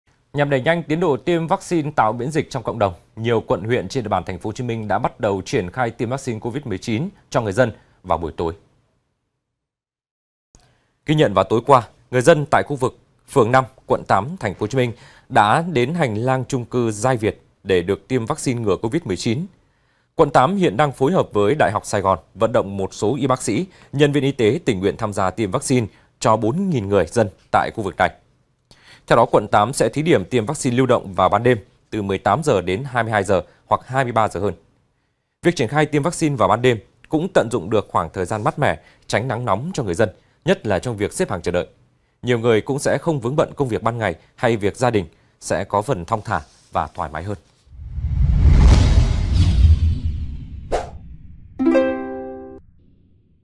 Người dân TP.HCM xếp hàng chờ tiêm Vắc-Xin trong đêm| VTC14
VTC14 |Nhằm đẩy nhanh tiến độ tiêm vắc-xin tạo miễn dịch trong cộng đồng, nhiều quận, huyện trên địa bàn TP. Hồ Chí Minh đã bắt đầu triển khai tiêm vaccine COVID-19 cho người dân vào buổi tối.